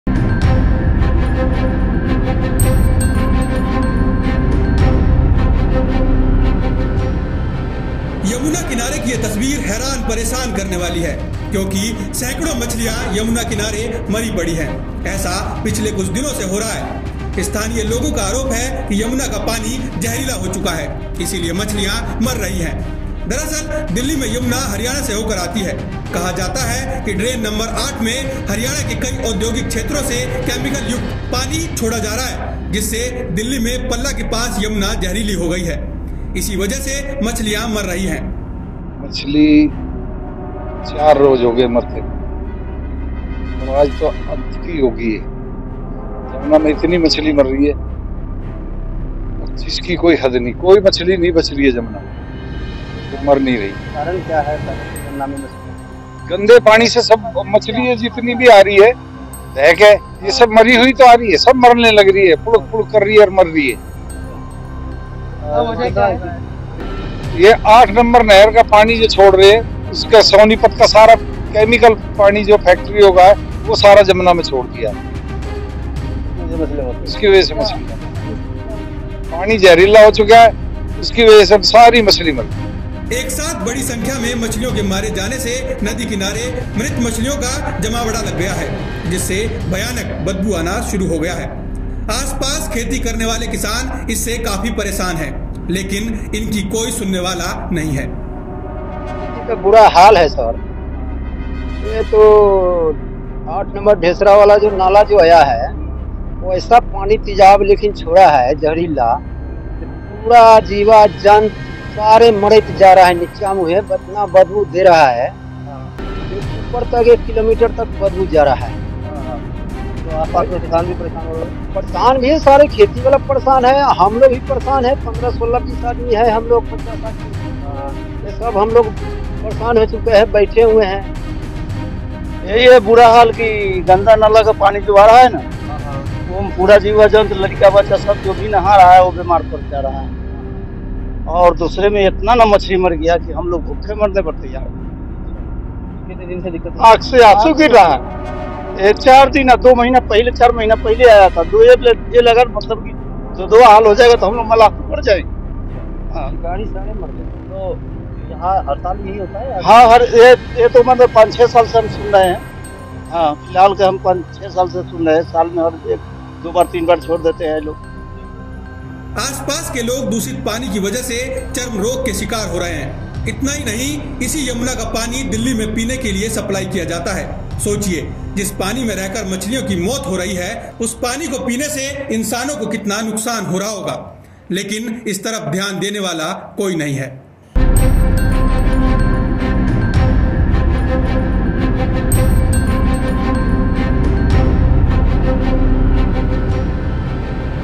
यमुना बनीं गंदा नाला, सैंकड़ों मछलियों की मौत ।।
दिल्ली में यमुना हरियाणा से होकर आती है, कहा जाता है कि ड्रेन नंबर 8 में हरियाणा के कई औद्योगिक क्षेत्रों से केमिकल युक्त पानी छोड़ा जा रहा है। जिससे दिल्ली में पल्ला के पास यमुना जहरीली हो गई है। इसी वजह से मछलियां मर रही हैं।
Yamuna in Delhi comes through Haryana, it is said that chemical laden water is being released from many industrial areas of Haryana in drain number 8. Due to which Yamuna has become poisonous near Palla in Delhi. Because of this the fishes are dying.